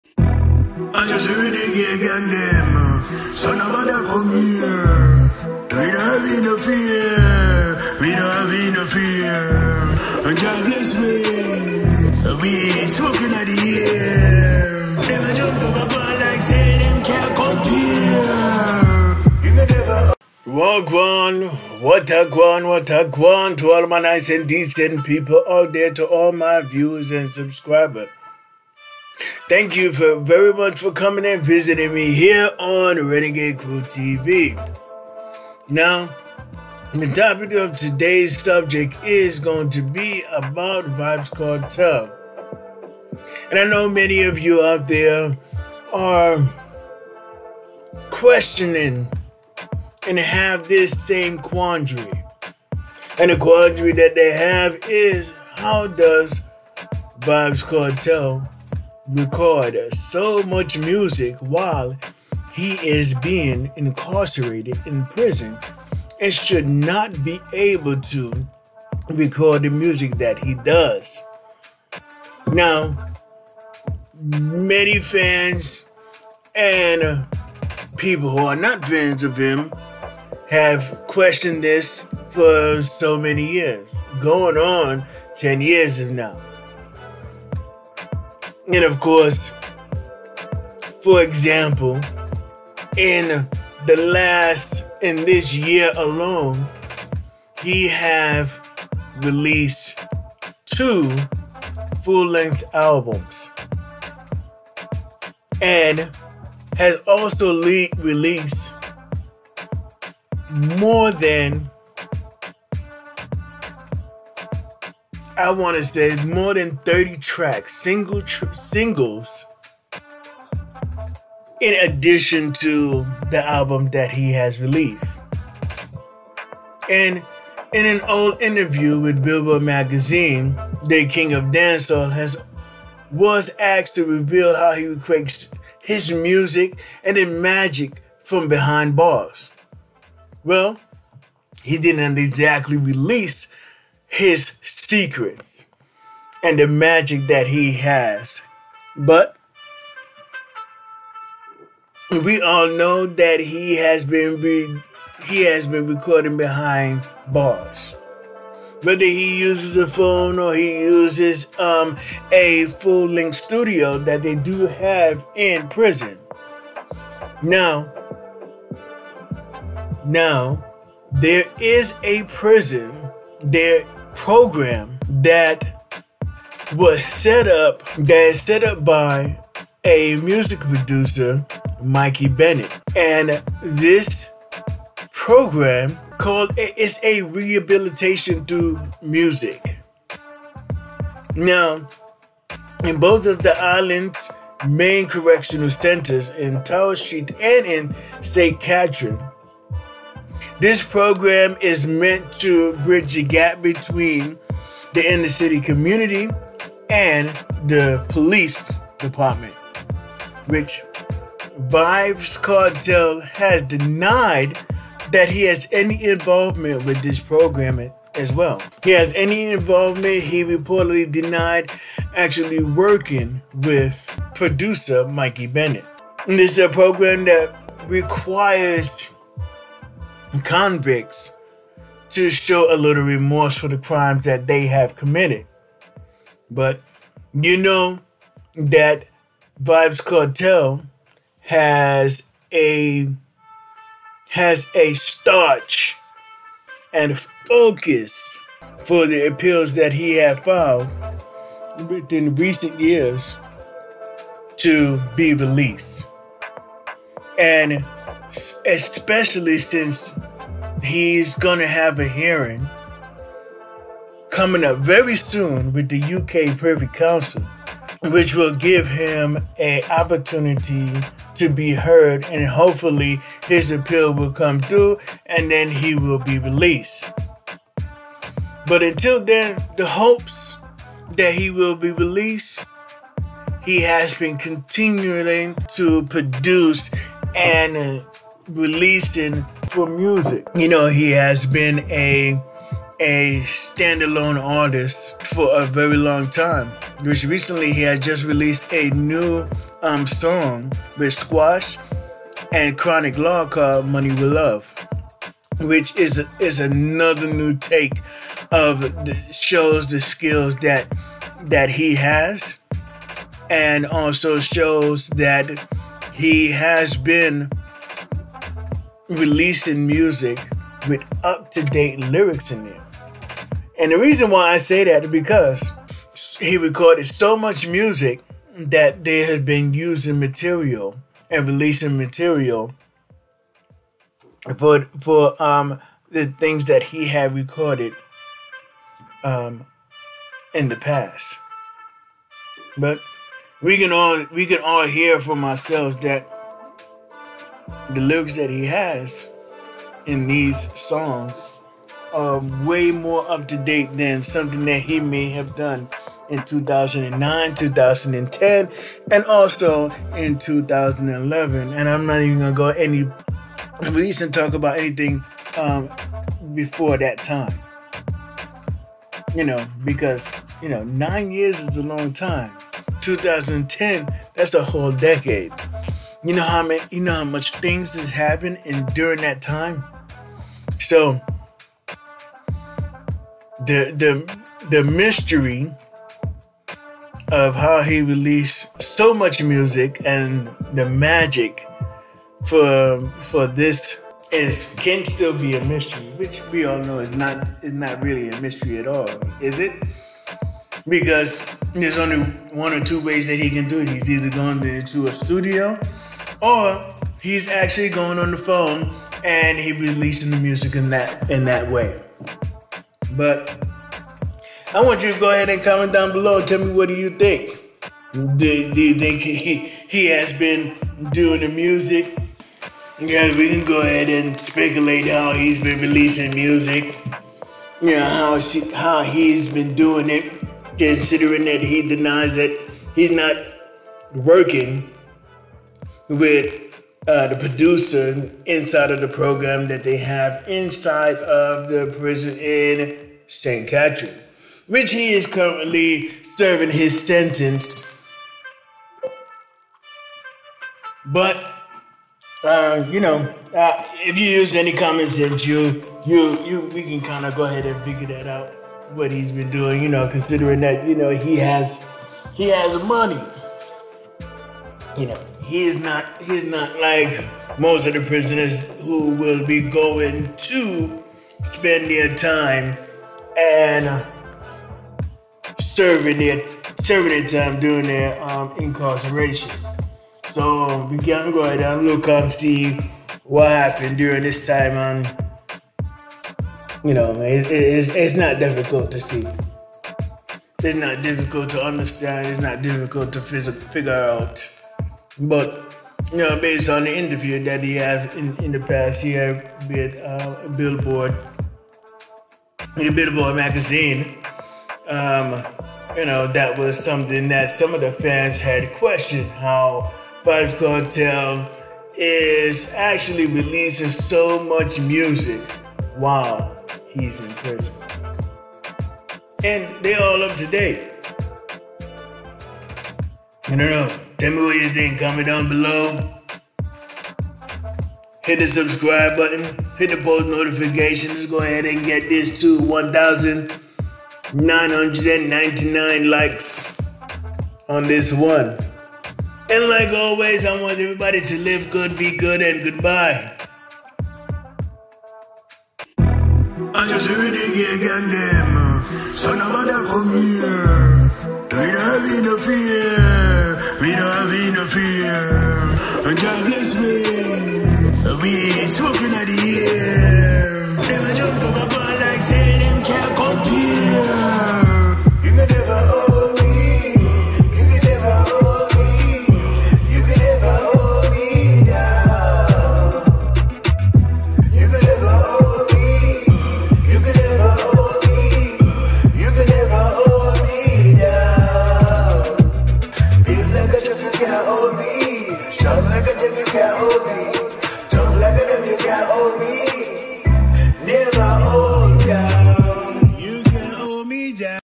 vybz Kartel How does Vybz Kartel record his music when he is in prison?
gaza Vybz Kartel How does Vybz Kartel record his music when he is in prison?

These are books that I wrote with my brother and look out for the third book coming soon called the foreigner.